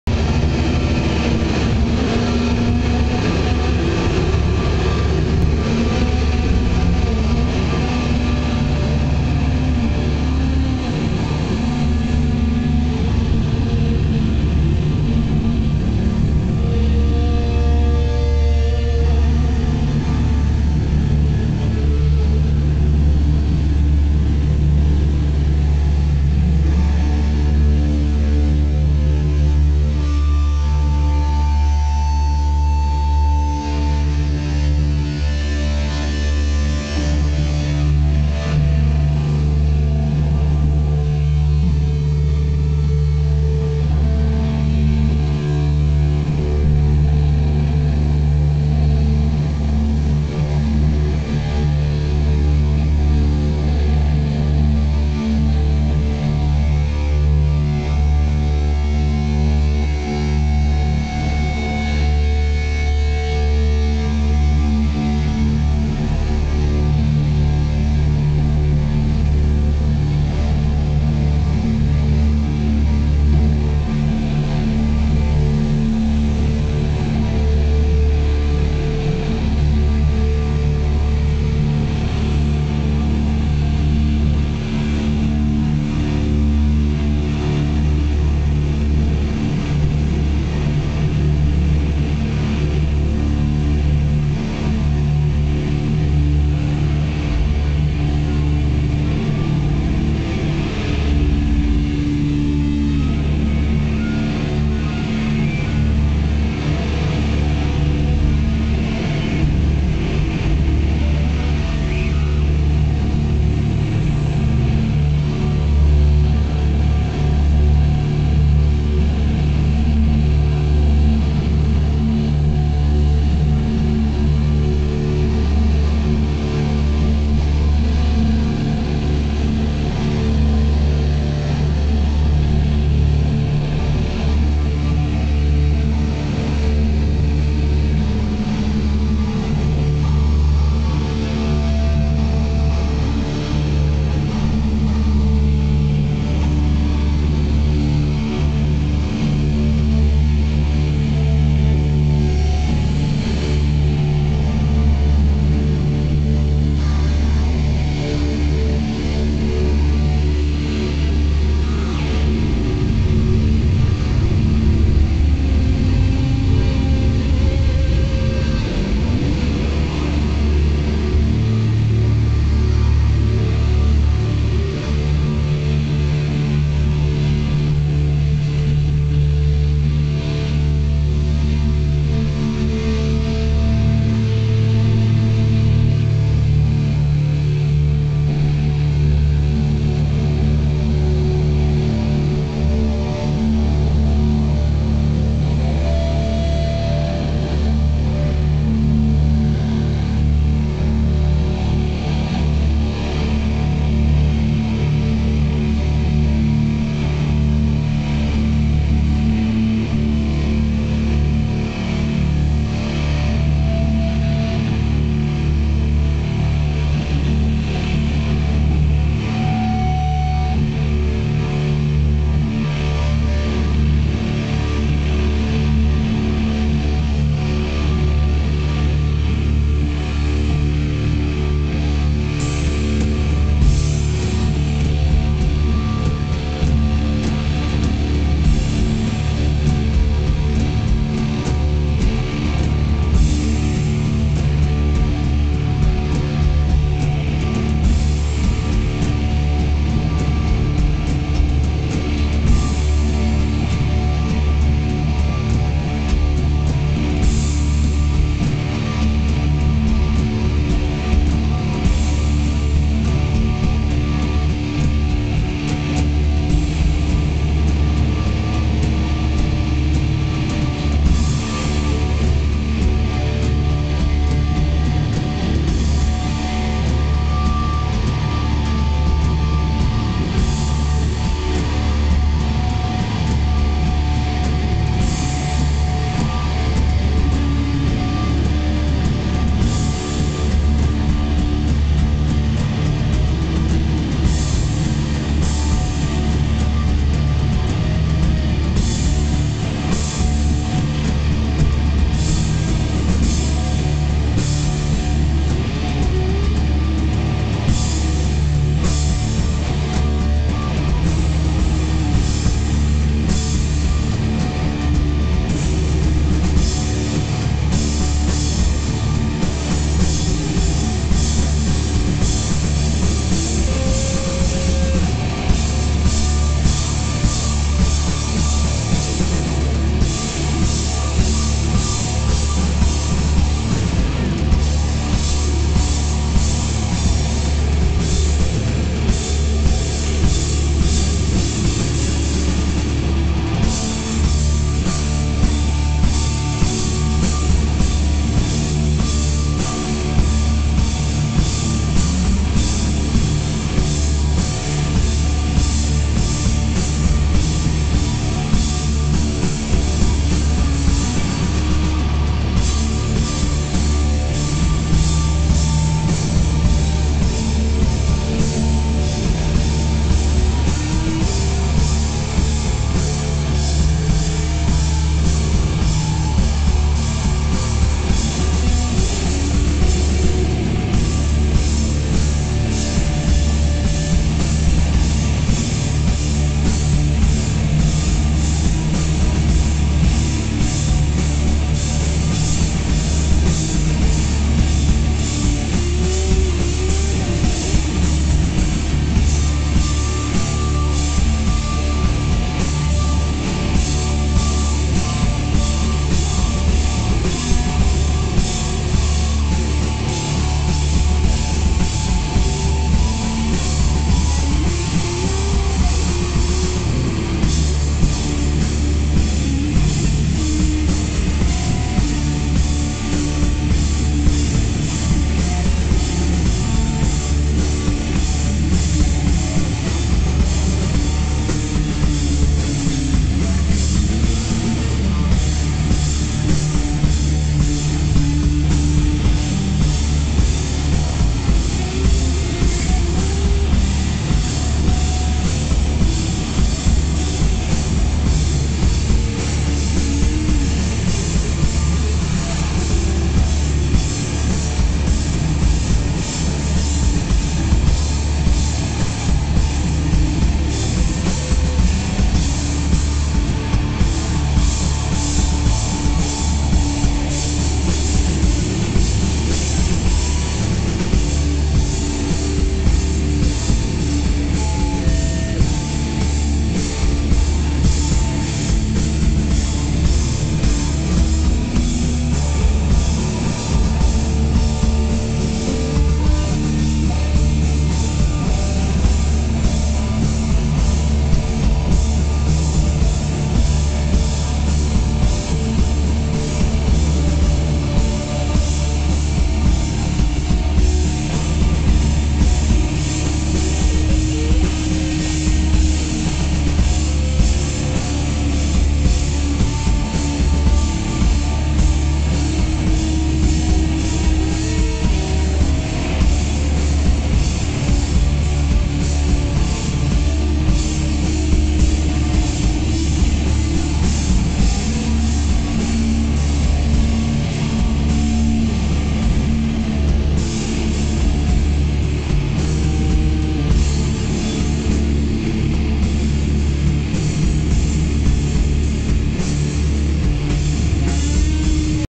Grey Daturas at Roadburn Fest 2009, Tilburg, Holland
The beginning portion of the Grey Daturas' set at Beyond the Pale (curated by Neurosis) Saturday at the Roadburn Festival in Tilburg, Netherlands.
25 April, 2009.
Grey Daturas, featuring Bonnie Mercer, Rob Mayson, and Robert MacManus.